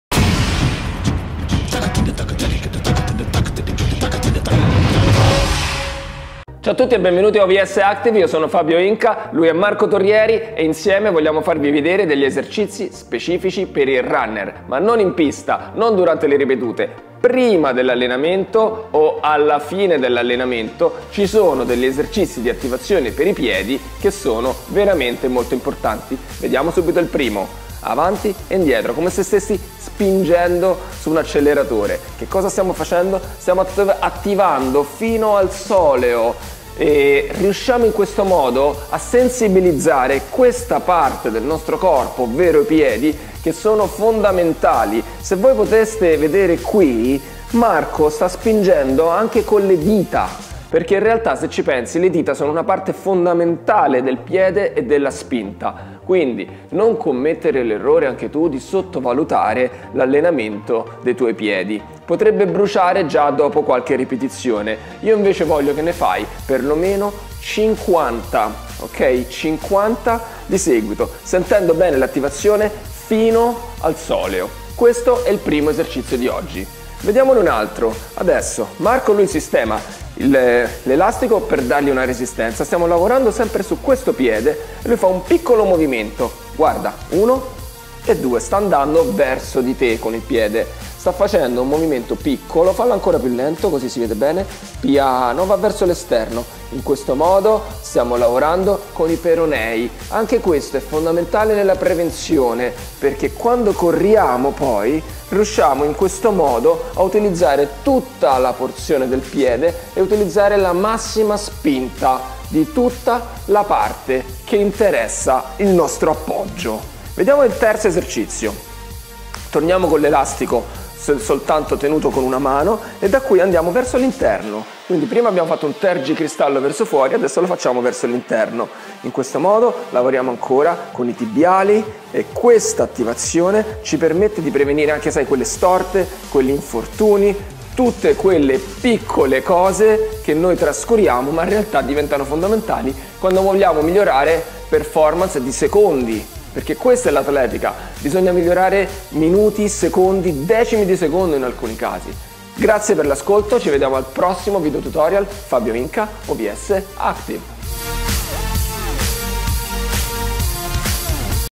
OVS ACTIVE: Attivazioni per i piedi
Con il campione Marco Torrieri, vediamo una serie di esercizi pensati per il runner, da fare prima di scendere in pista o a fine dell’allenamento. Un lavoro di attivazione per i piedi che arriva fino al soleo e coinvolge le dita. Mai sottovalutare l’allenamento dei piedi!Fai 50 ripetizioni di seguito anche se brucia! Poi fai lavorare i peronei e alla fine i tibiali per prevenire storte e infortuni e migliorare la tua corsa.